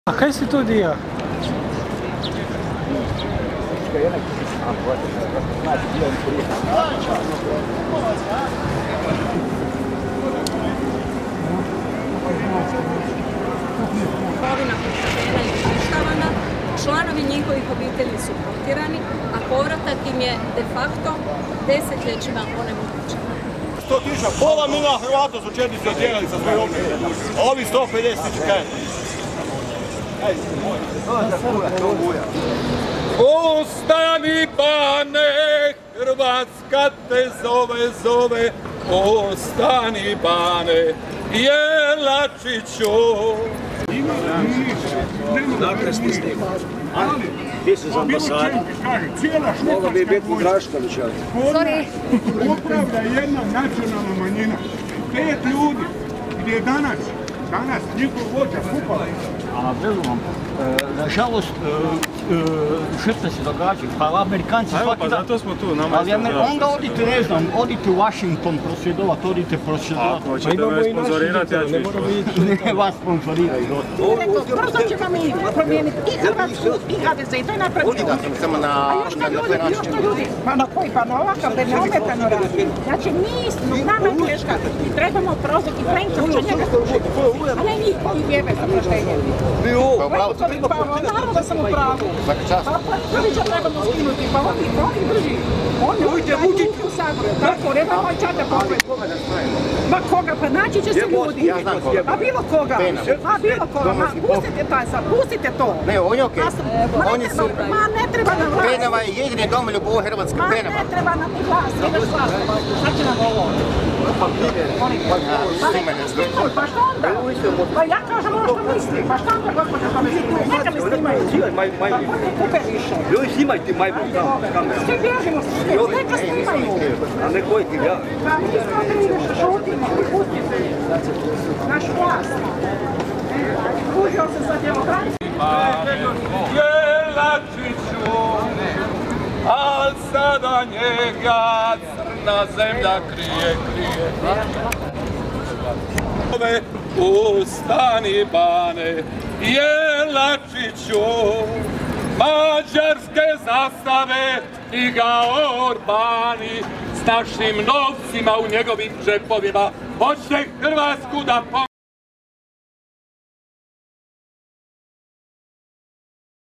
Na Trgu bana Josipa Jelačića održana antiratna akcija, prolaznici reagirali
Na Trgu bana Josipa Jelačića okupila se skupina na antiratnoj akciji na dan obljetnice vojne operacija Oluja.